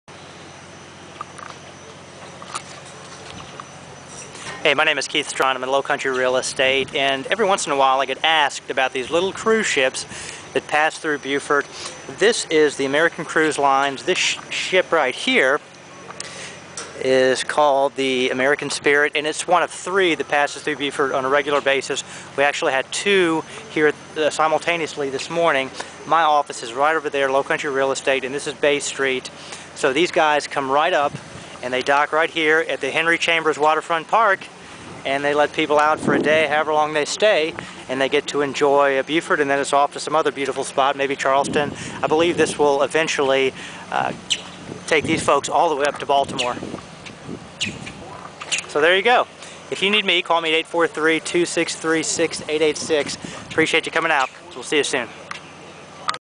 Beaufort, SC Weather Update for May 6, 2009
American Cruise Lines pays a visit to Beaufort, SC Thursday afternoon. For more information on Beaufort, SC, or the Beaufort, SC real estate market,